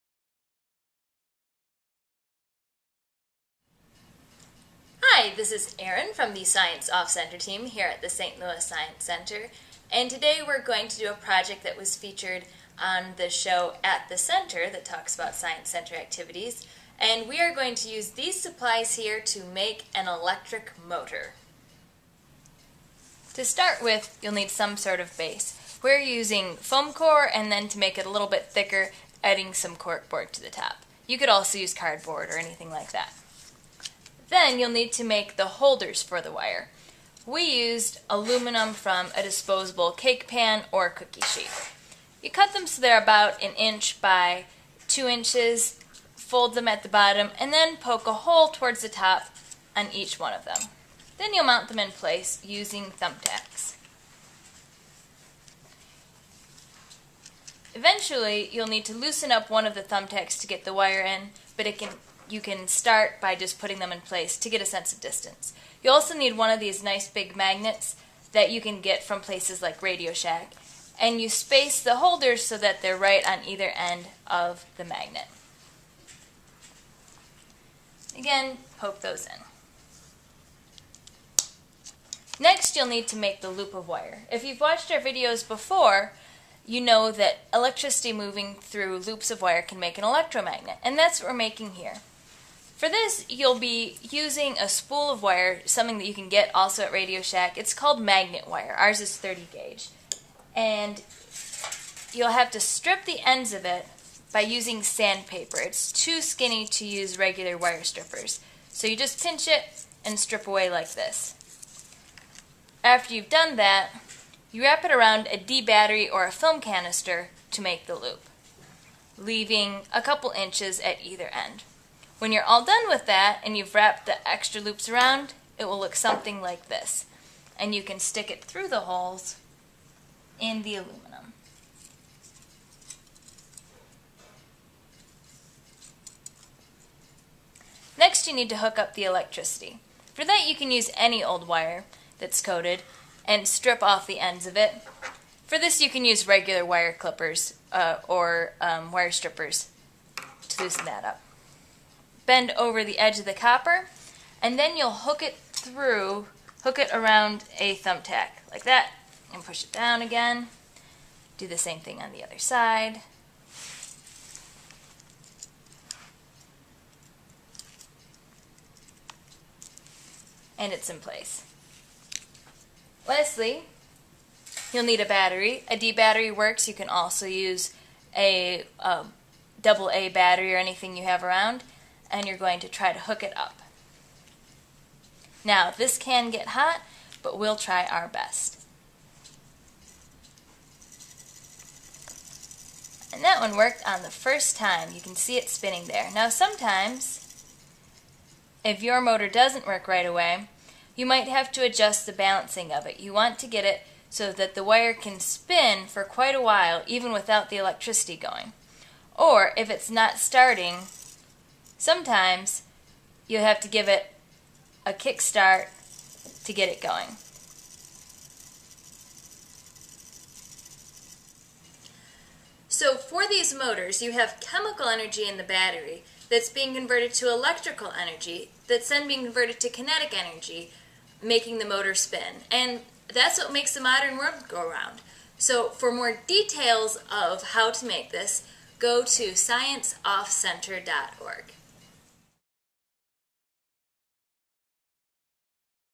Electric Motor: a fun, at-home science experiment
Learn how to make your very own electric motor with items around your house! For a detailed description of how and what is going on here, visit us